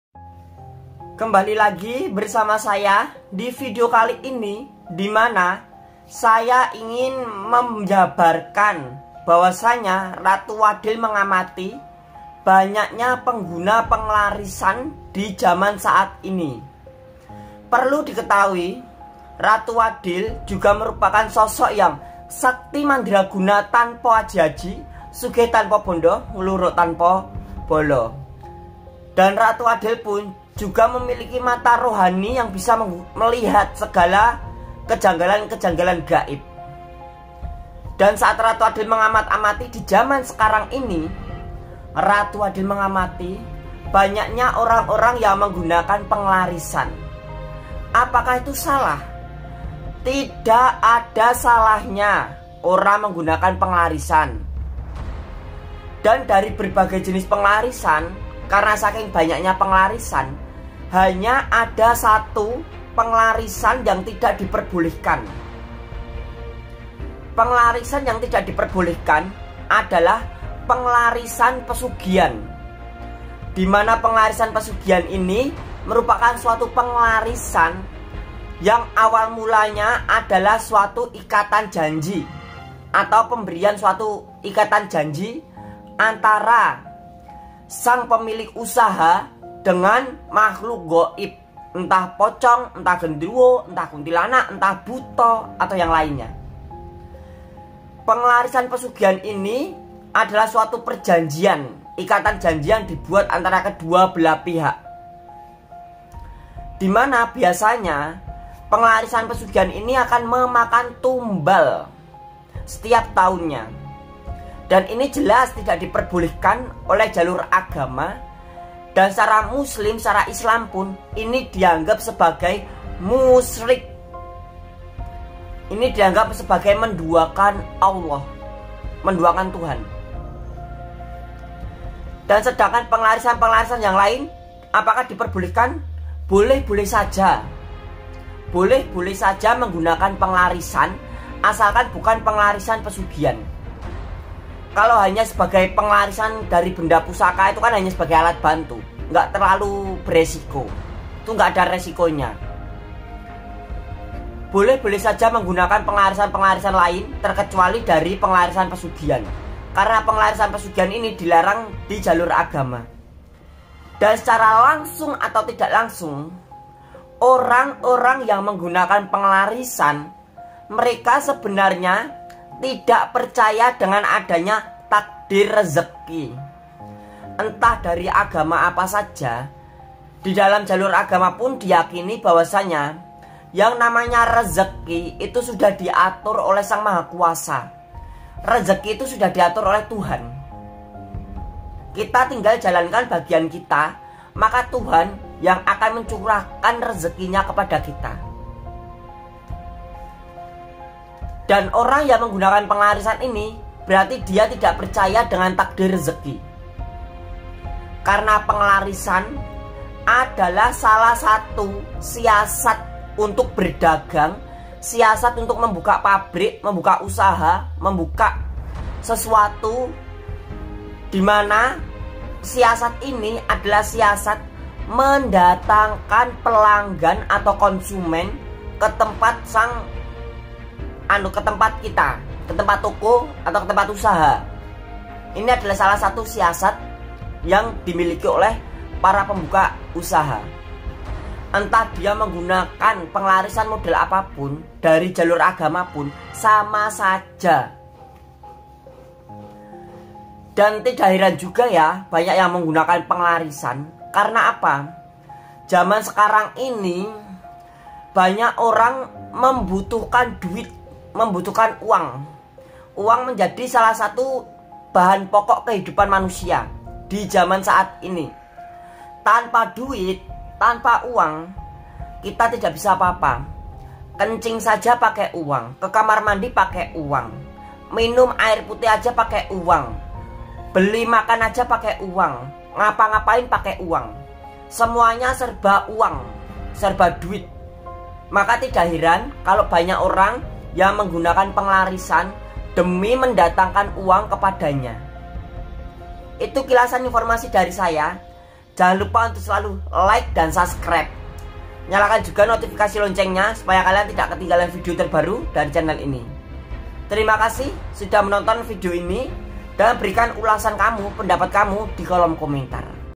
Ratu Adil Amati banyaknya pengguna penglarisan‼️@FaktaDuniaGaib
Vidio ini menyatakan pengamatan ratu adil mengenai pengguna penglarisan yang begitu banyaknya dizaman sekarang ini.
Ratu Adil melihat banyak yang gunakan penglarisan karena meraih uang untuk dirinya.
Dan begitu luar biasanya uang bagi manusia sekarang ini.

Semoga vidio ini bermanfaat menambah wawasan kamu semua.

Nyalakan juga notifikasi loncengnya.

Berikan komentar kamu dikolom komentar ya.